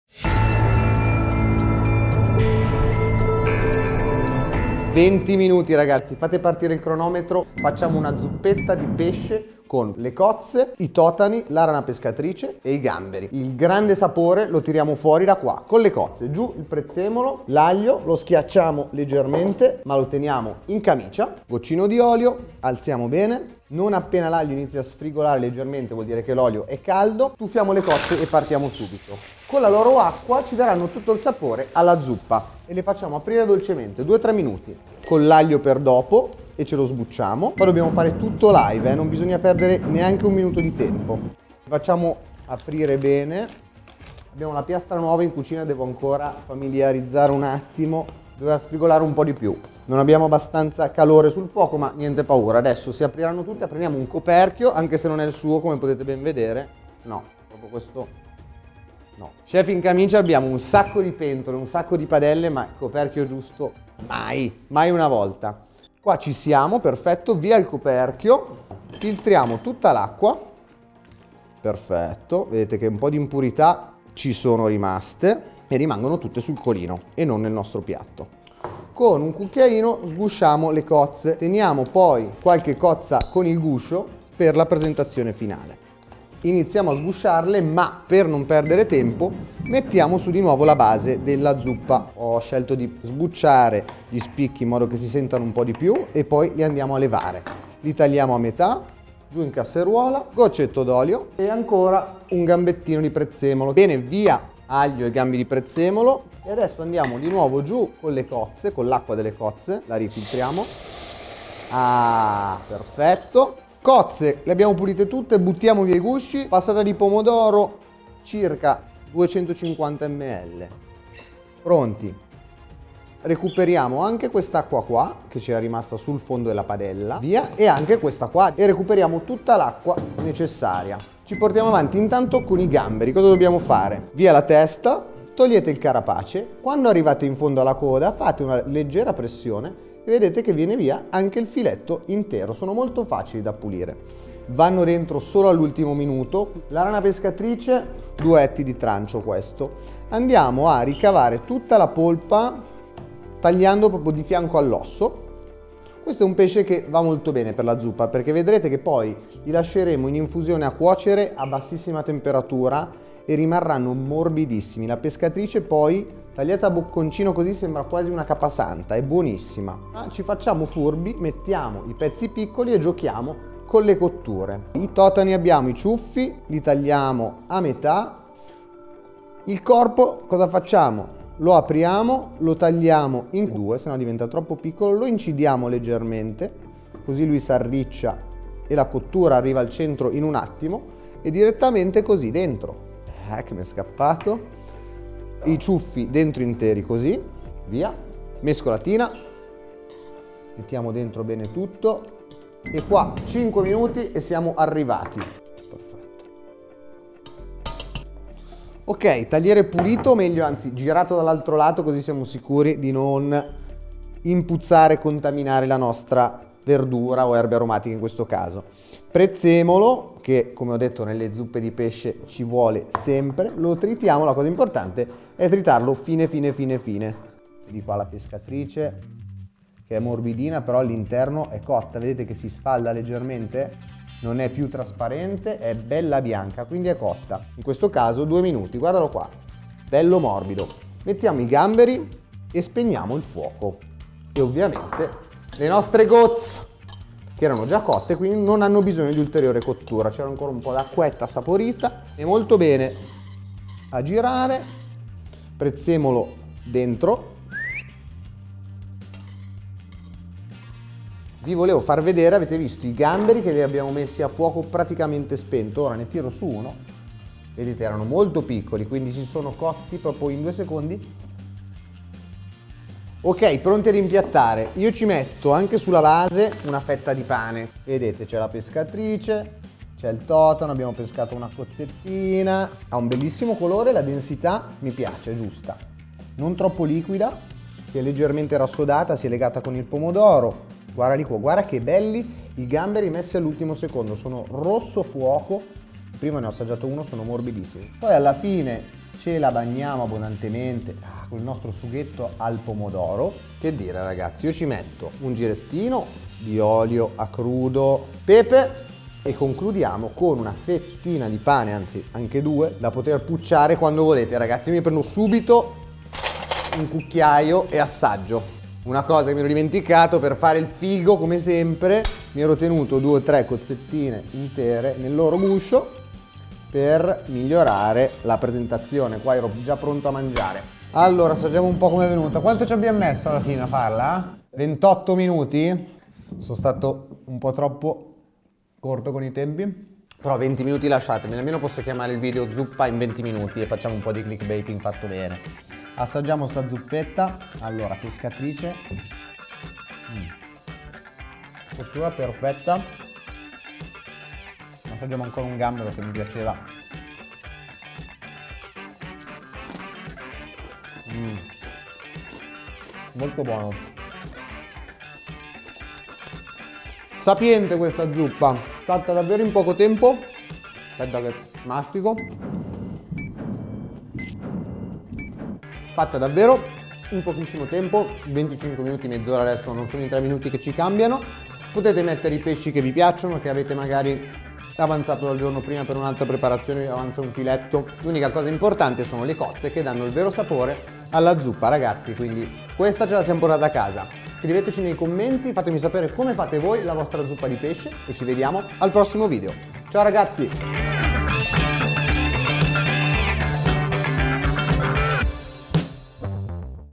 Zuppa di pesce pronta in soli 20 MINUTI!!! *VELOCISSIMA*
Un piatto gustoso pronto in pochissimo tempo. Da non credere vero? Guardate il video!